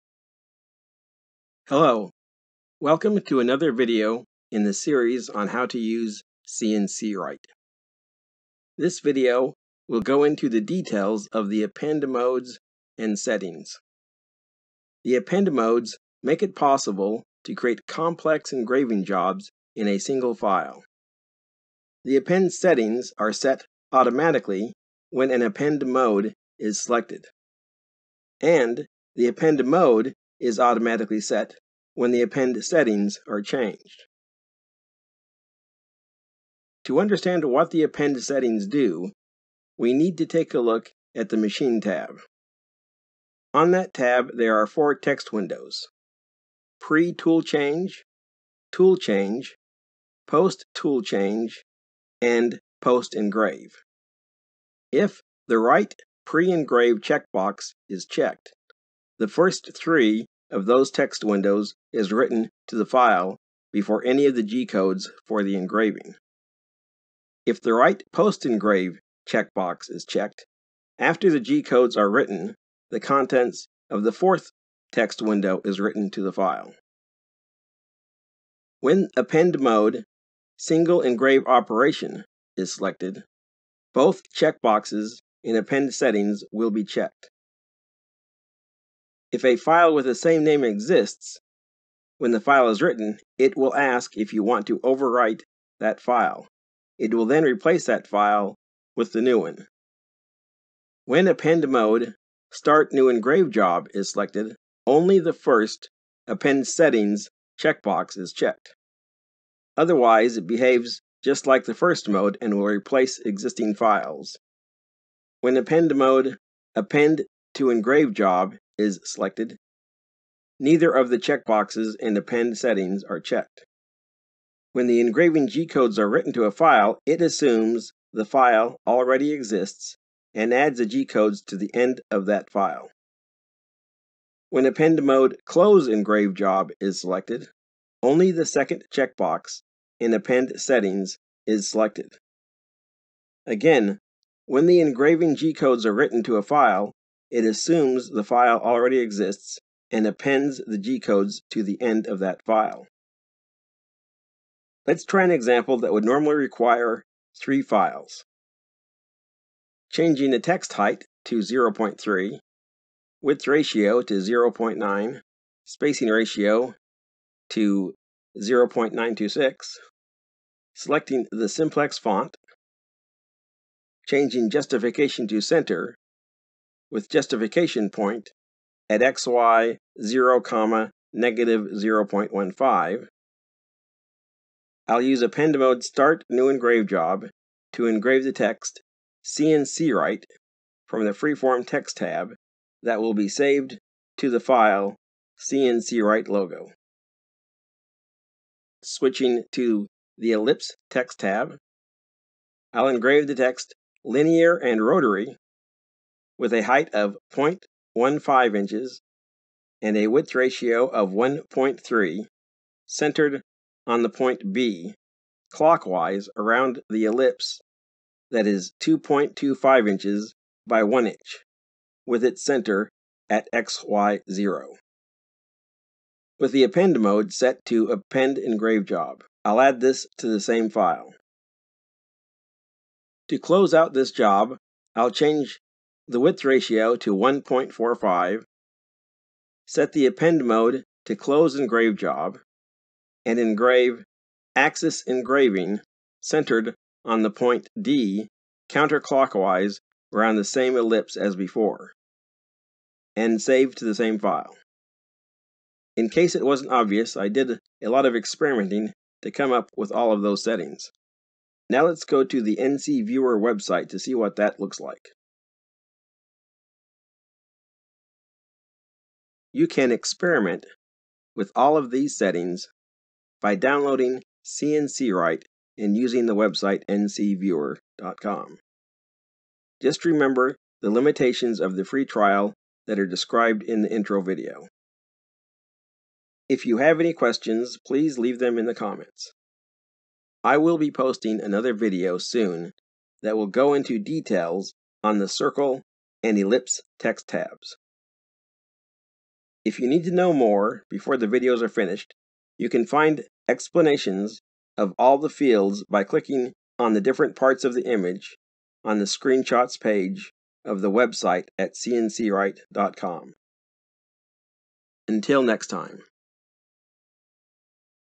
CNC Write Append Modes and Settings
This video describes in detail the Append Modes and Settings.  It also gives a preview of the "Ellipse Text" tab.

Feel free to experiment with all of these settings by downloading CNC Write from the website linked below.  And using the website NCViewer.com to see what the output G-Code will do.